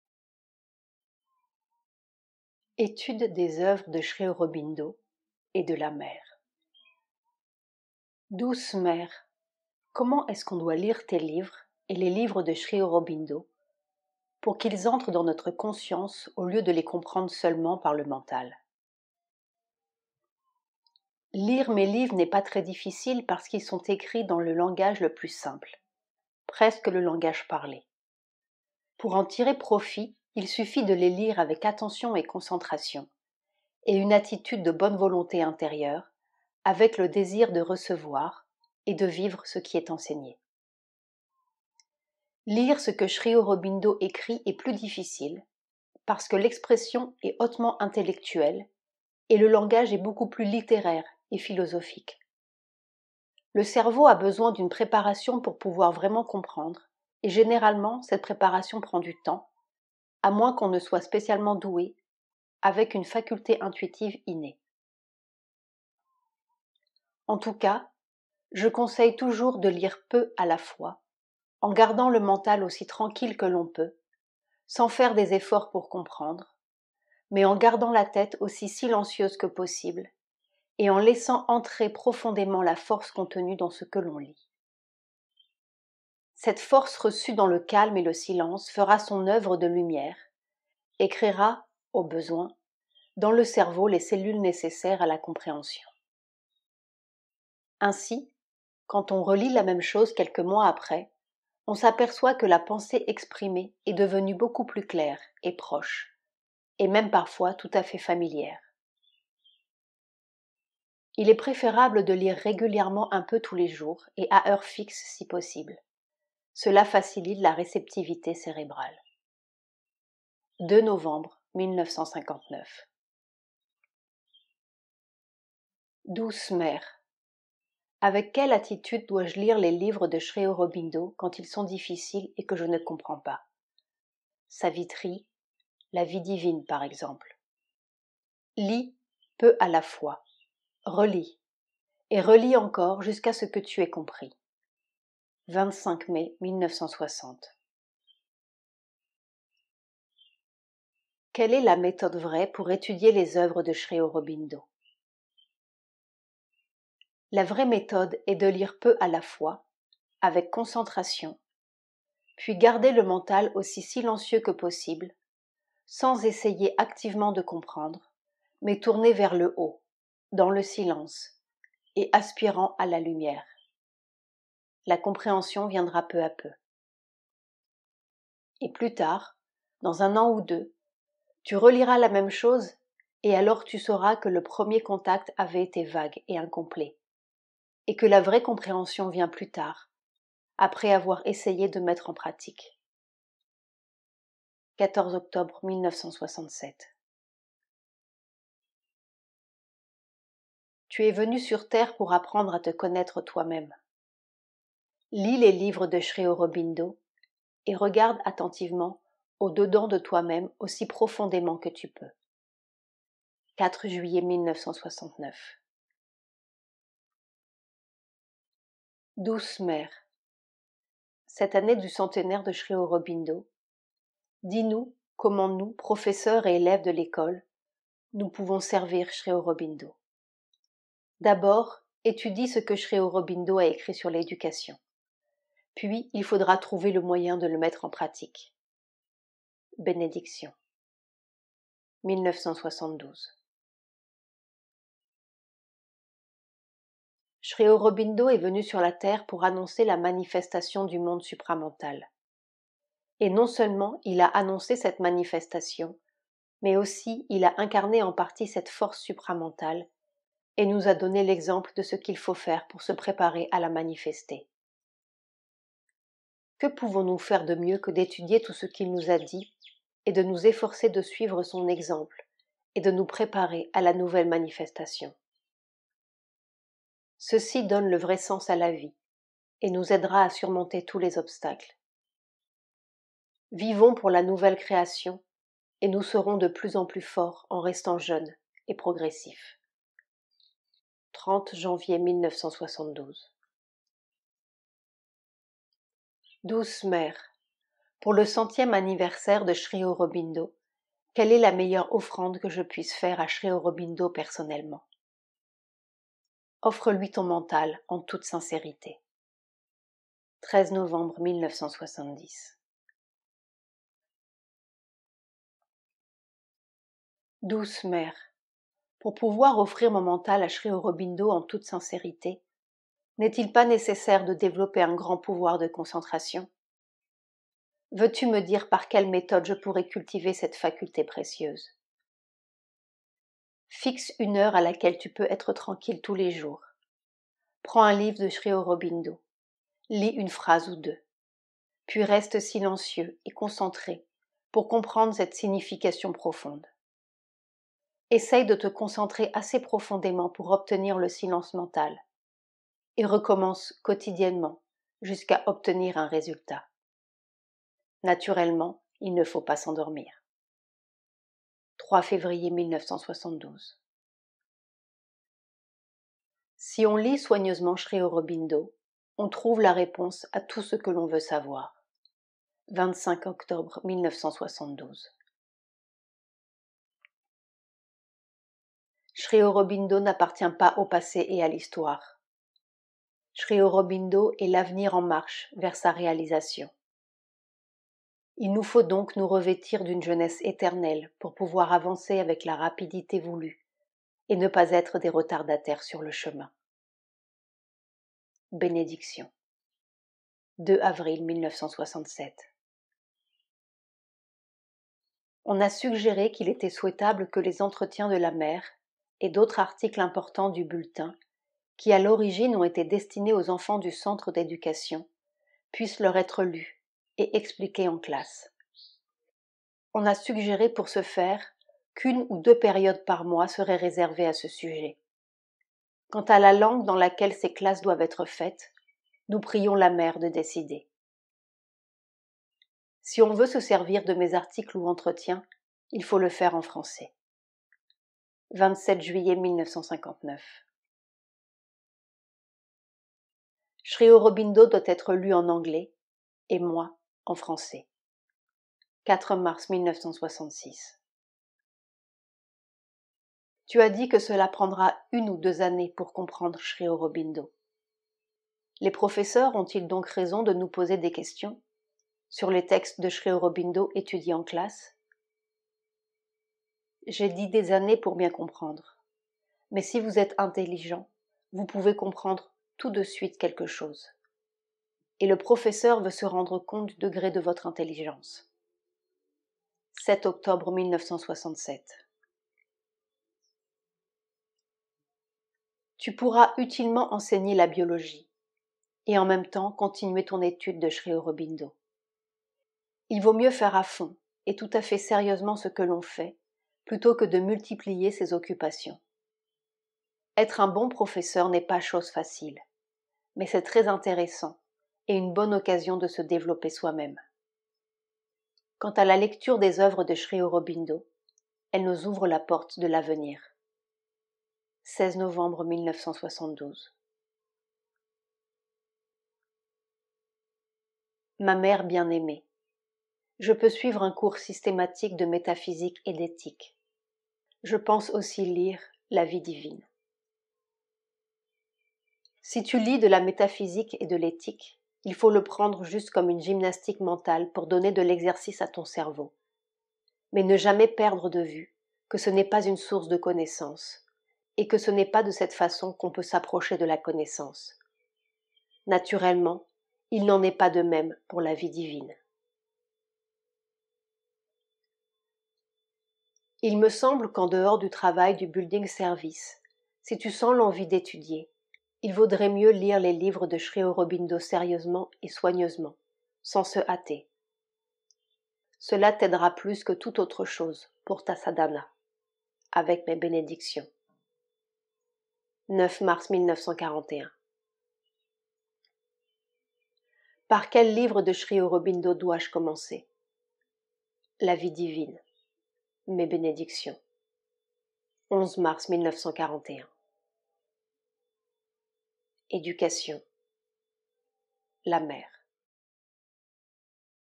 Étude des œuvres de Sri Aurobindo et de la Mère, Extrait | Éducation, La Mère
Oeuvre de Mère à lire et à écouter, pour les élèves petits ou grands.
Dans ce volume ont été réunis des articles, des messages, des lettres et des conversations de la Mère avec des étudiants et des professeurs de l’école de l’Ashram, et trois pièces de théâtre : Vers l’Avenir, Le Grand Secret et L’Ascension vers la Vérité.

❊ Pour retrouver toutes les oeuvres de Sri Aurobindo, La Mère et Satprem

🔹 Atelier
Sur la voie du Yoga Intégral : Informations et inscription